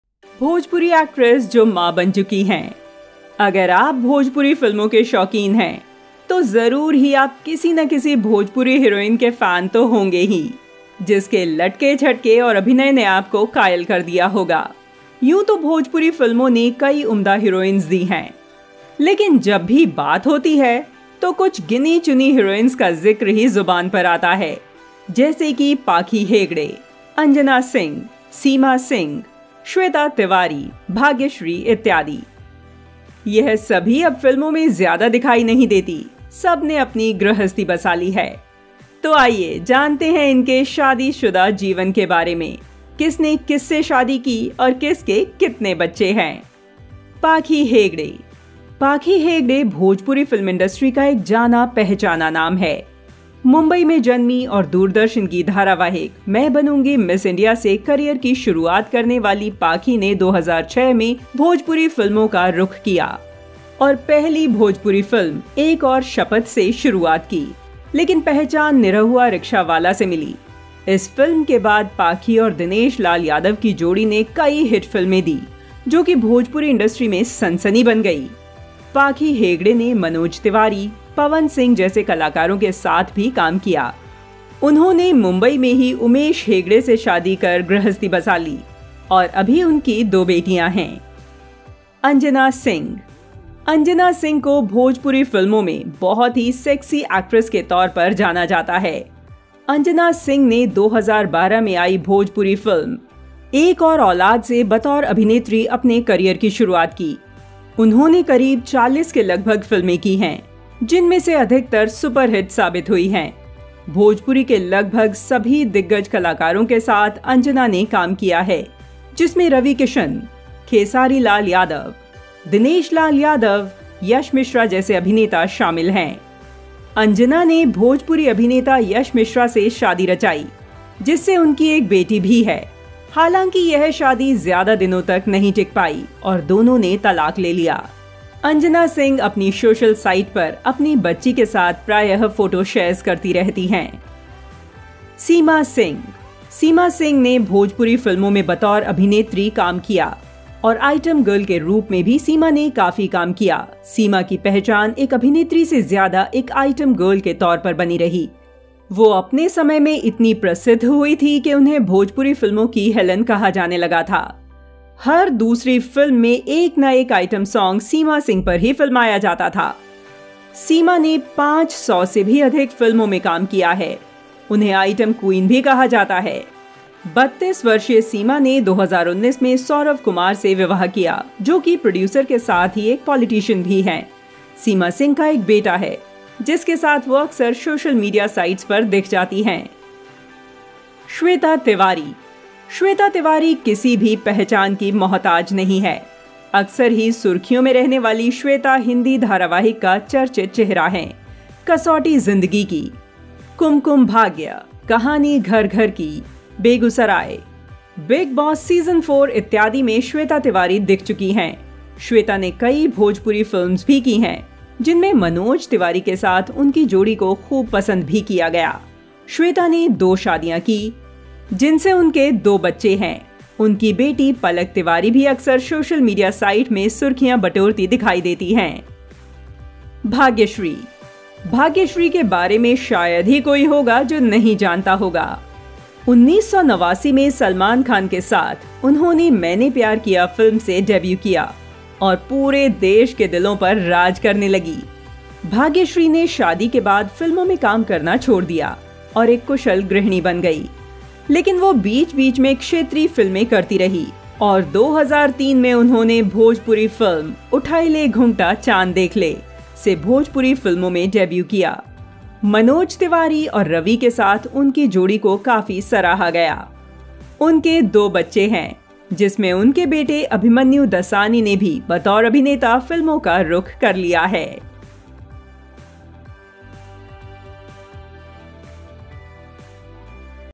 भोजपुरी अभिनेत्रियां जो माँ बन चुकी है ! Bhojpuri Actress Become A Mother
Credit & Thanks : -  Pakhi Hegde, Shweta Tiwari, Seema Singh & Anjana Singh.

Produce By. Bhojpuriyanews.Com
Voice Over : Kirti Chauhan
Story : Niharika Singh
Edited By: Amit Kannaujiya & Aditya Singh